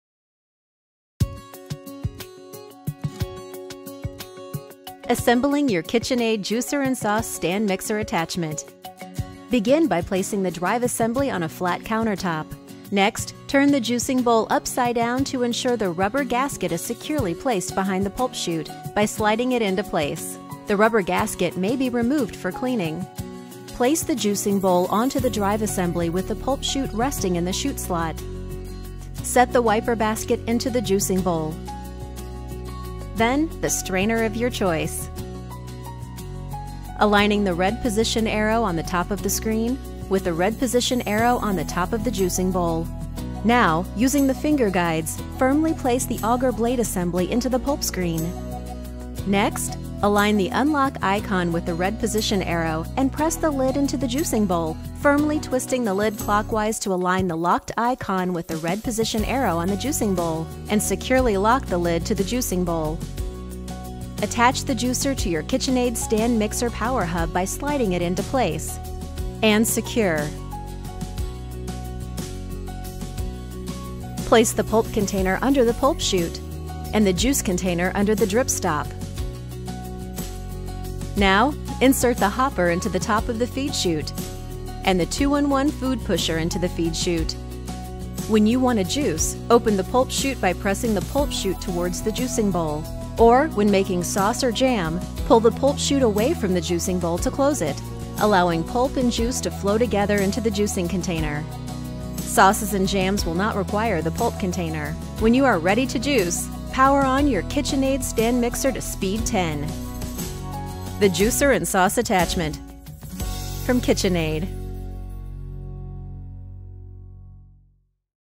Mixer Artisan KitchenAid cu accesoriu pentru suc si sosuri.
Designul unic, constructia robusta din metal si functiile tipice unui aparat profesional transforma acest mixer intr-un element indispensabil  si perfect integrat in orice bucatarie, gata oricand sa execute cele mai dificile sarcini.Nimic nu este prea greu pentru mixerul Artisan®: puteti amesteca diferite tipuri de aluaturi (moi, medii si tari), toca si prepara carne, legume si fructe, rula si taia paste, pregati smoothies, sucuri naturale si chiar inghetata. Totul se realizeaza printr-o simpla atasare a unuia dintre accesoriile potrivite.